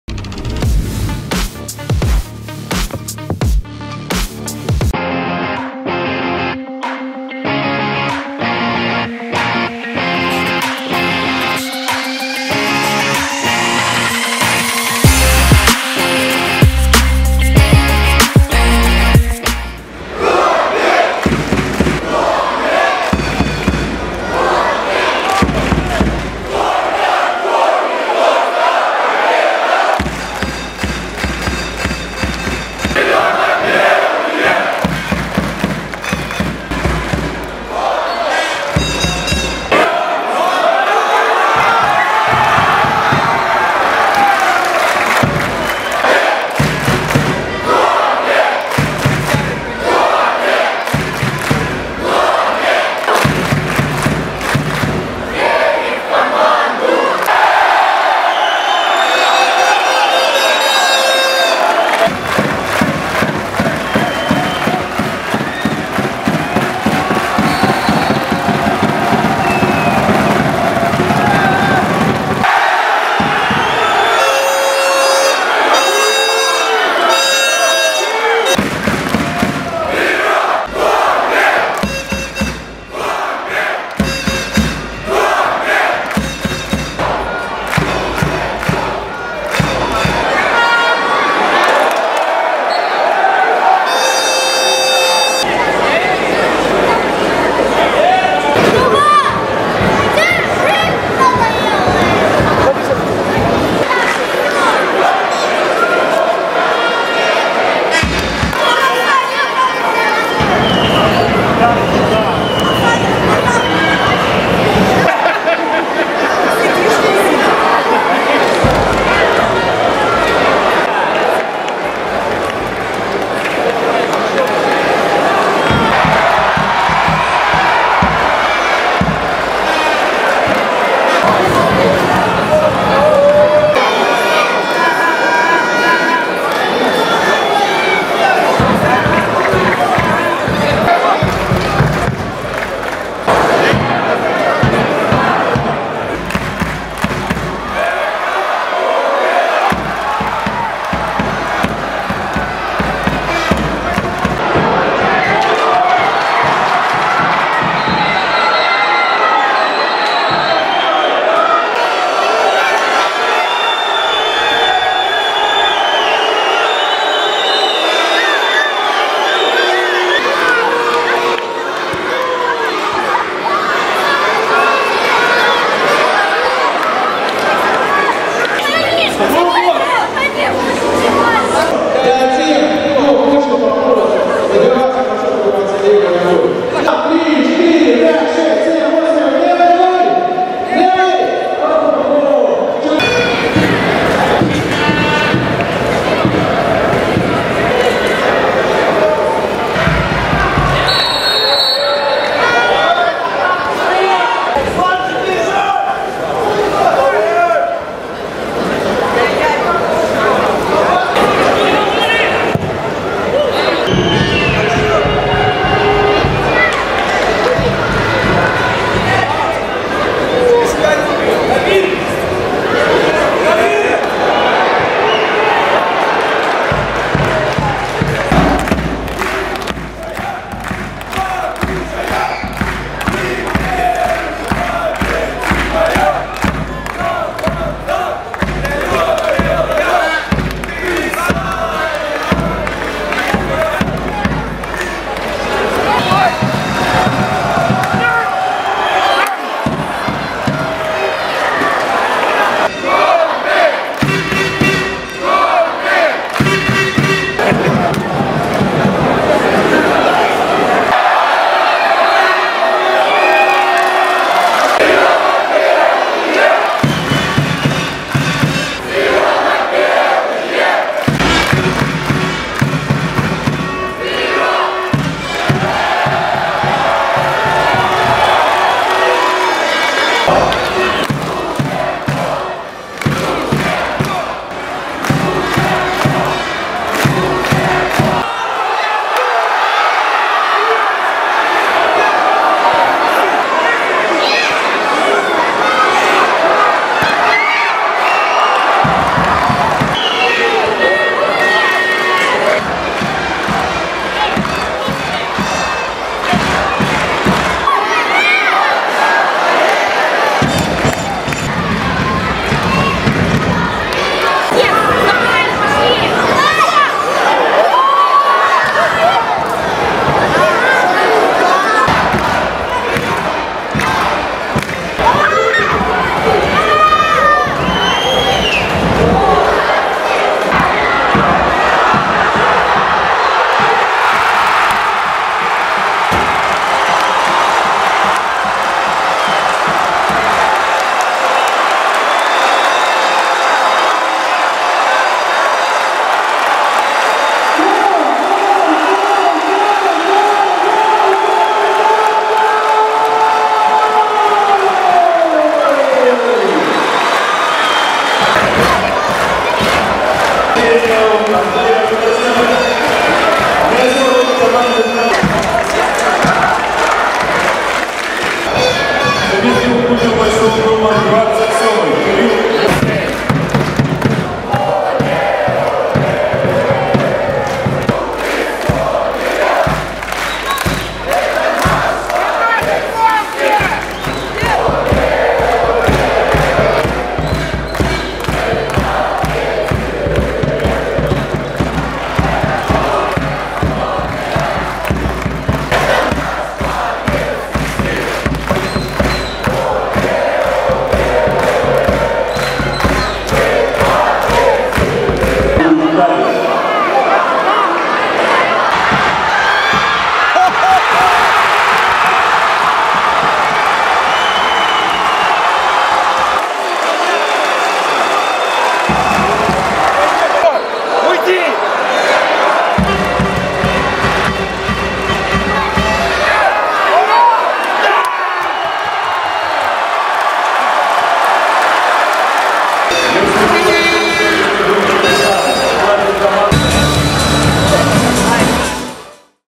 Атмосфера домашнего матча с «Динамо-Брест» (27.09.2024)
Атмосфера победного матча с брестским «Динамо» ― в нашем видеорепортаже.